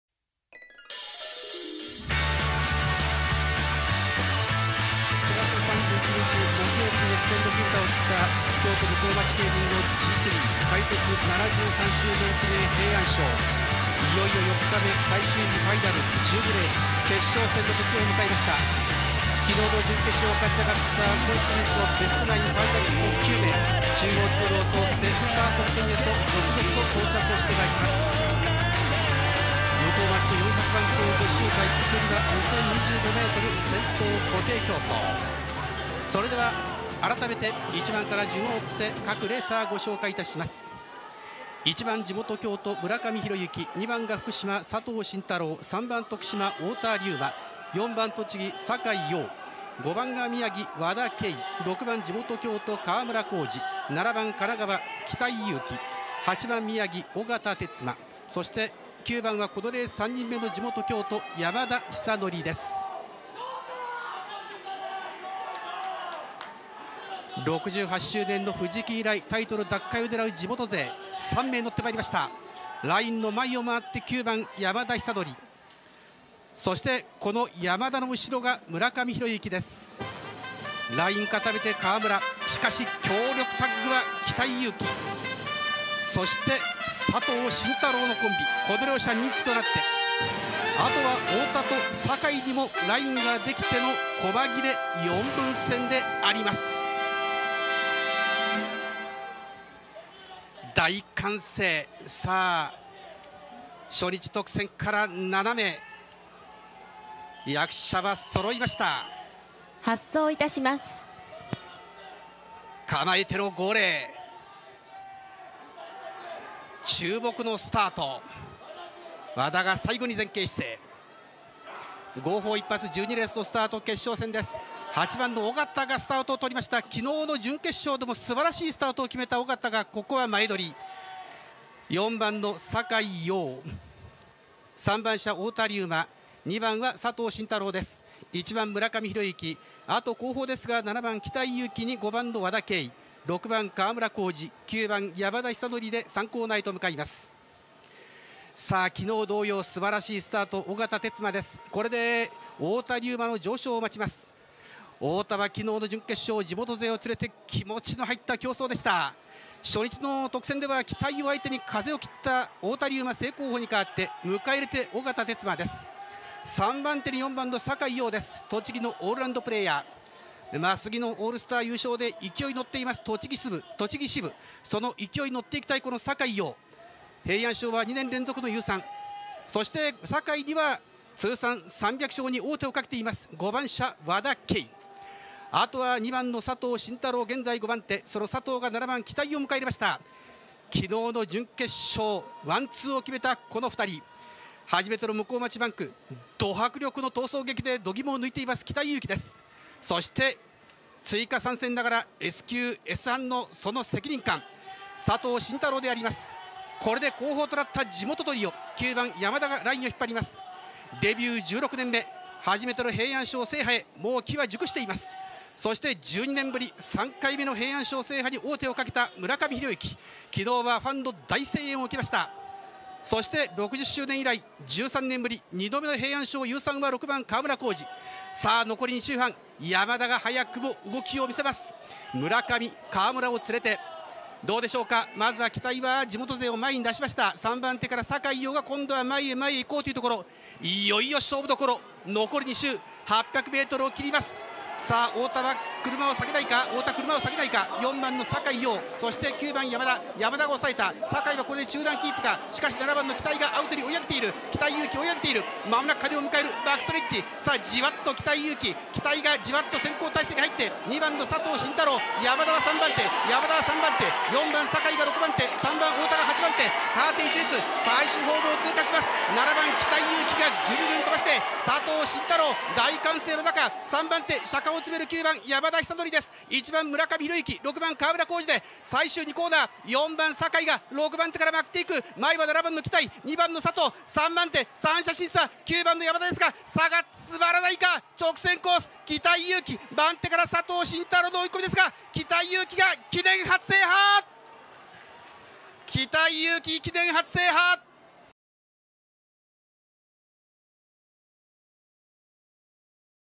【北井佑季】向日町記念決勝！圧巻の逃げ切りで初優勝
2023-9-3
京都向日町記念決勝
優勝
北井佑季選手
記念初優勝おめでとう